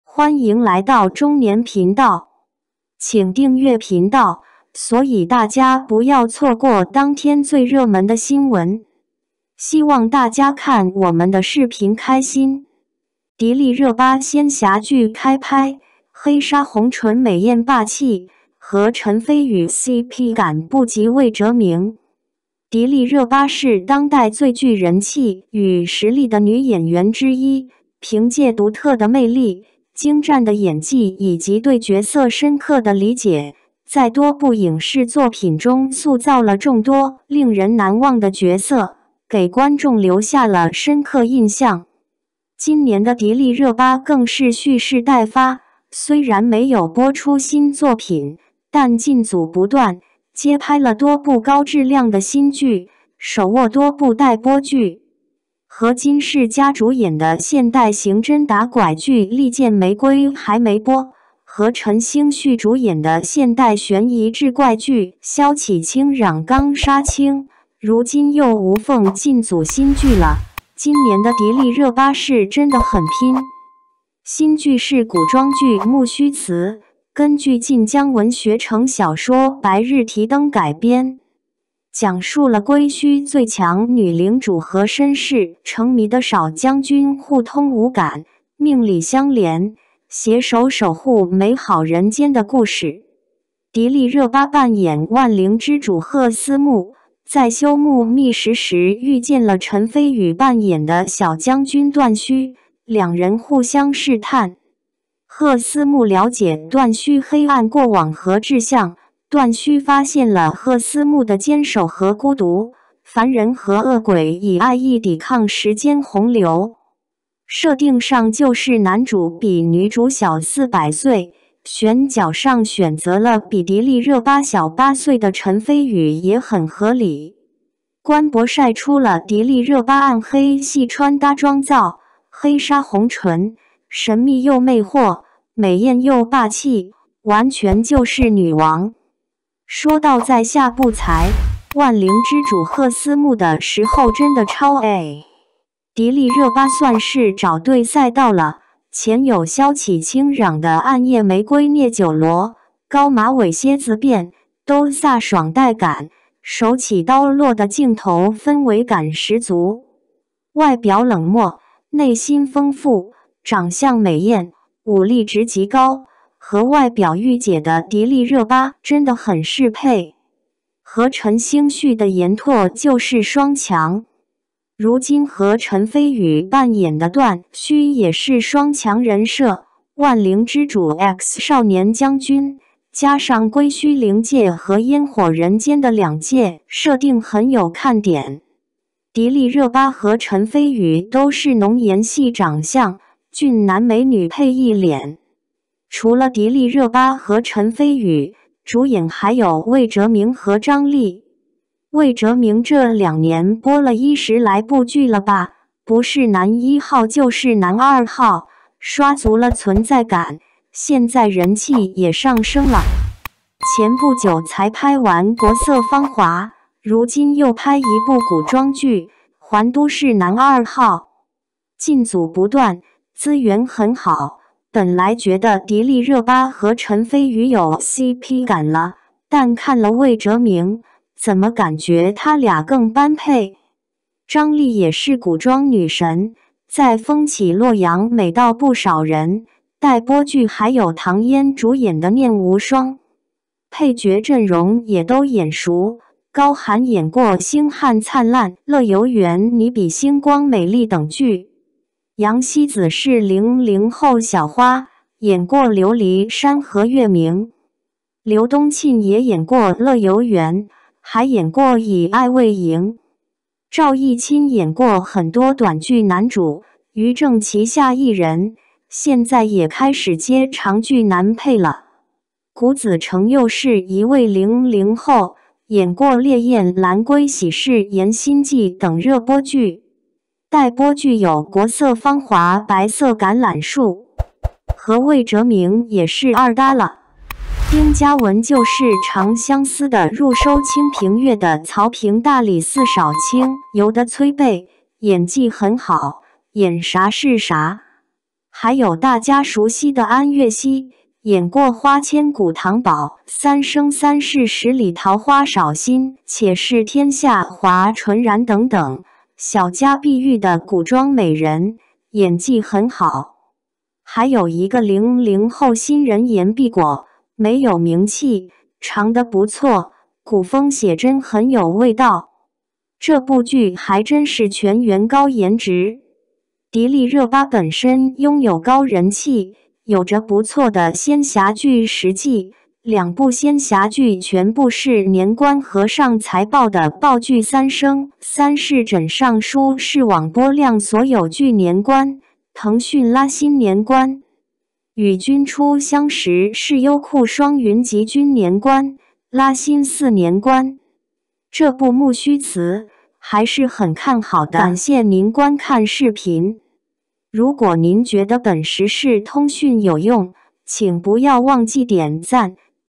【中年9月7日】迪丽热巴 仙侠剧开拍，黑纱红唇美艳霸气，和陈飞宇cp感不及魏哲鸣#dilireba
迪丽热巴仙侠剧开拍，黑纱红唇美艳霸气，和陈飞宇cp感不及魏哲鸣